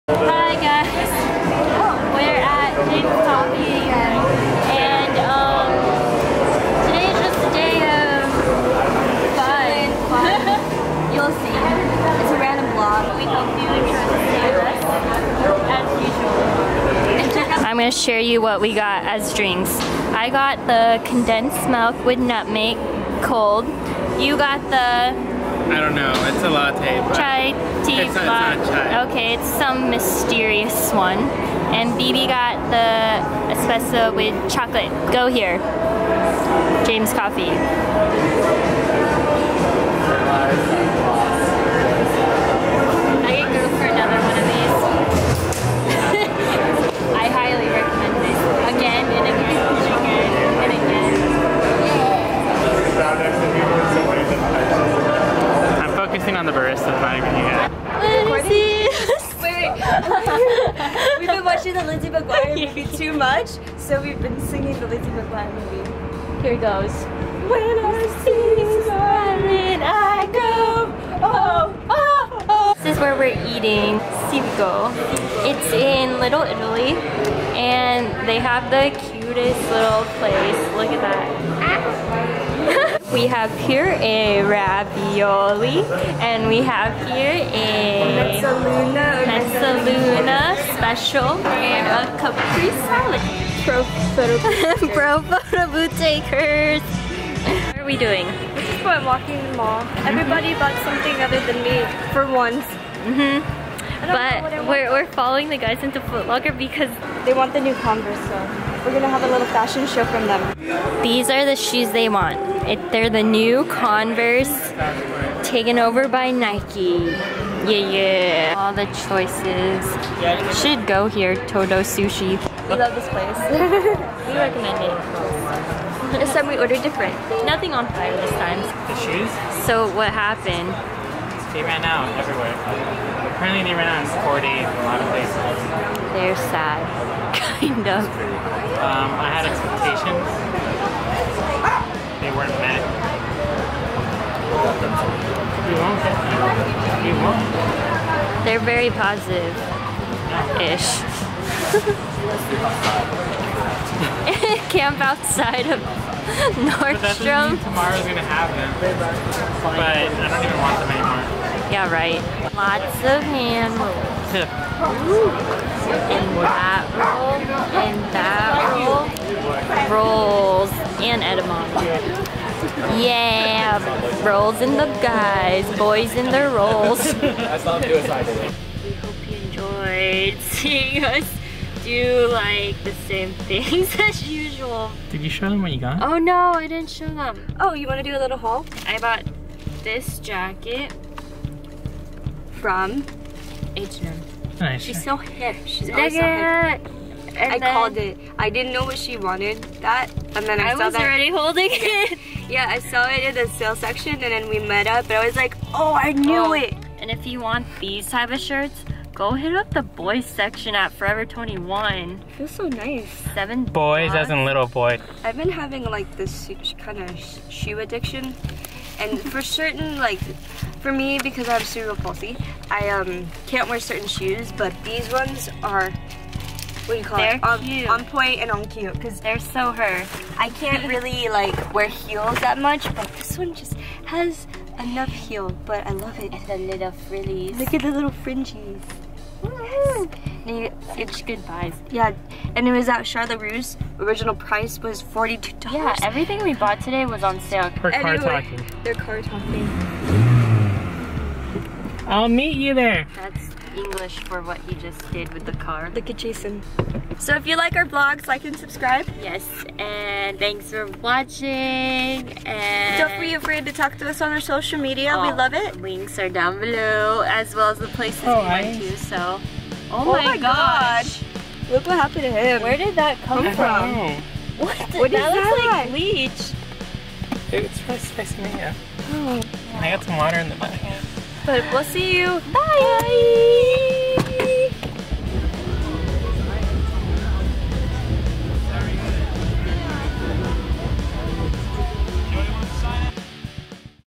James Coffee, Little Italy, Converse Fail & Todo Sushi
We hope you enjoyed spending the day with us!
If you're in the San Diego area check out the places we mentioned in the video down below :)

Eucalyptus Square, 9823 Carroll Canyon Rd # K, San Diego, CA 92131
We'd love to talk to you guys!!
Have a rad day!